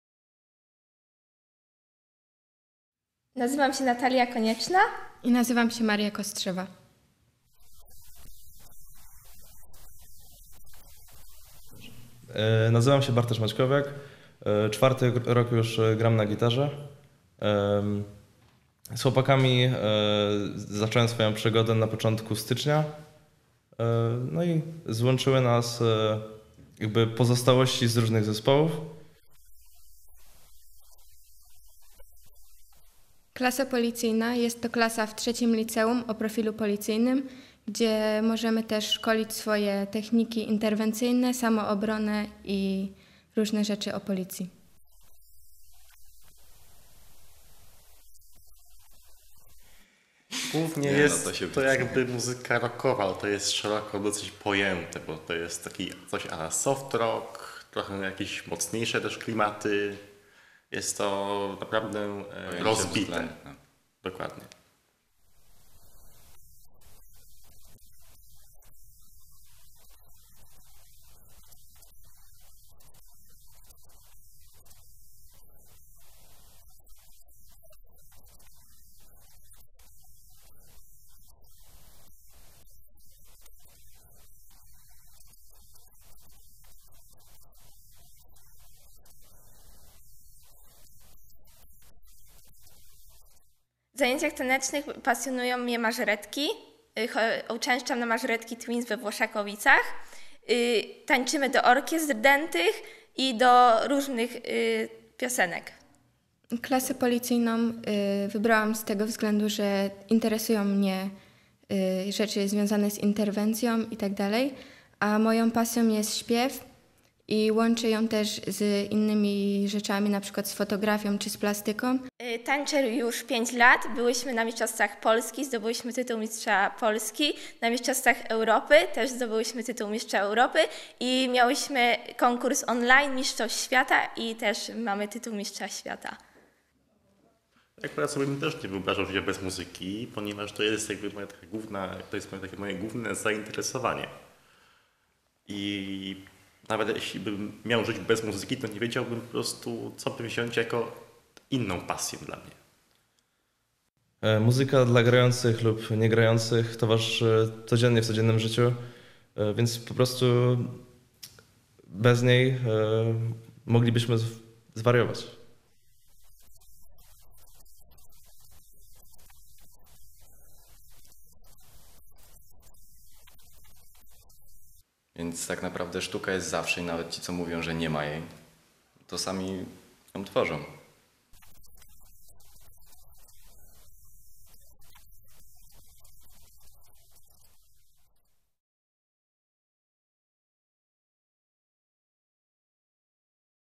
Sztuka to poczucie radości.
Świat bez sztuki byłby szary i smutny i nudny. Nie wyobrażają sobie życia bez muzyki, czy sztuki w ogóle, bo sztuka daje  poczucie radości. Tak mówią, My bardzo prosimy dorosłych, żeby o ty,m pamiętali.